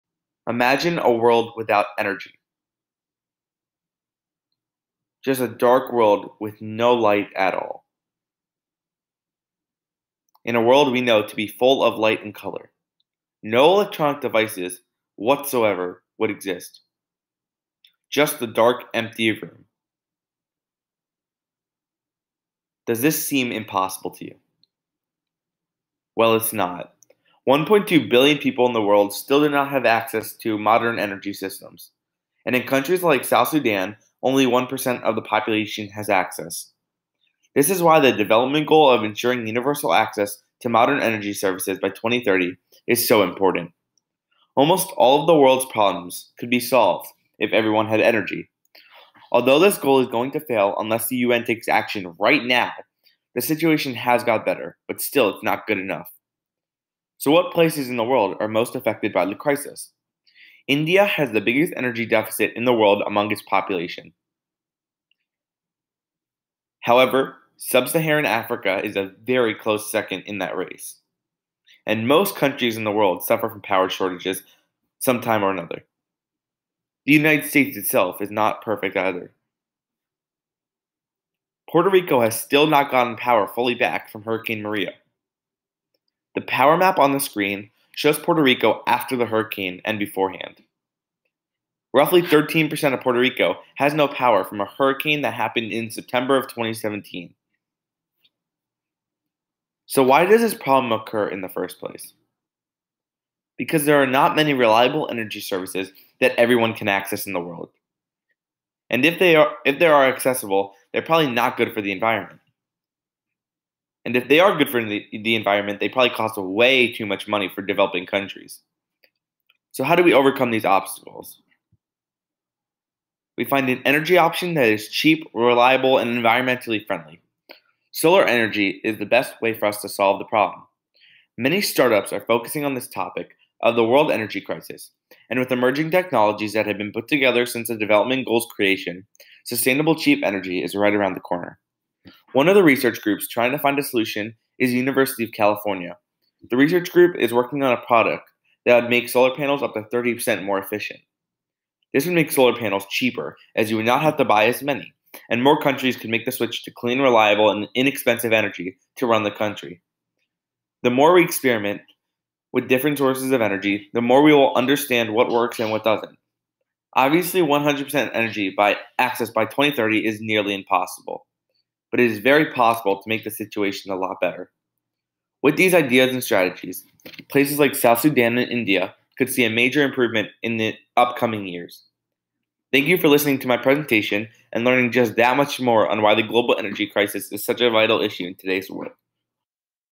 Global Energy Crisis
A video made by Jake Goldberg, Bullis School '20, for Dr. Romeyn's Contemporary Global Issues Class. This video addresses the UN's Sustainable Development Goal #7, Affordable and Clean Energy